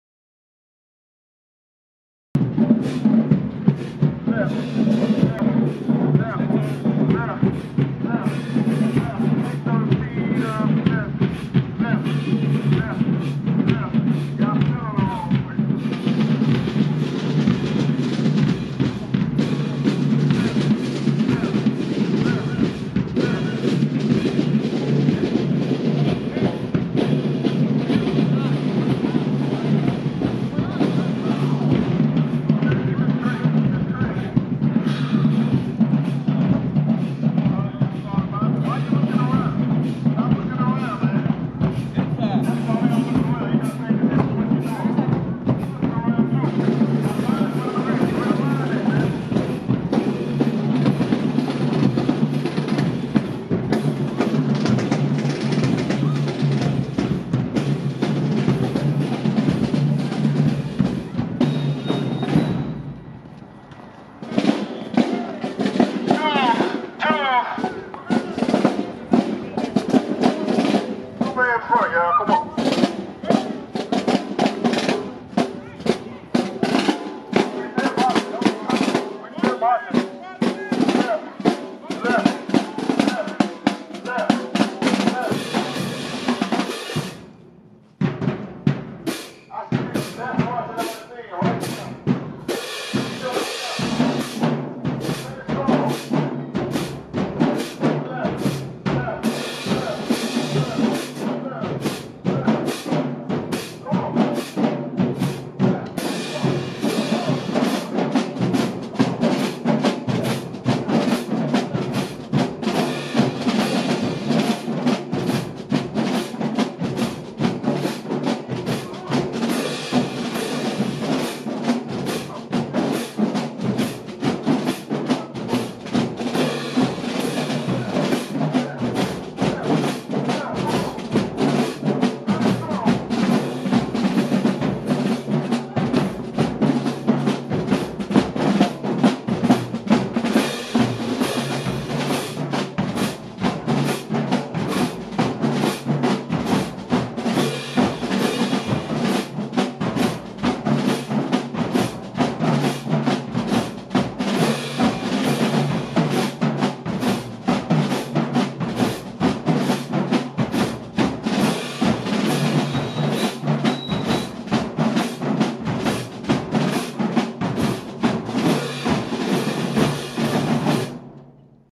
Langston Hughes Marching band 2023 marching back into school (HD) 4K
Official video highlights featuring the Langston Hughes marching band performance. Highlighting all marching band programs.  A day with the Langston Hughes marching band. Be sure to watch in HD format.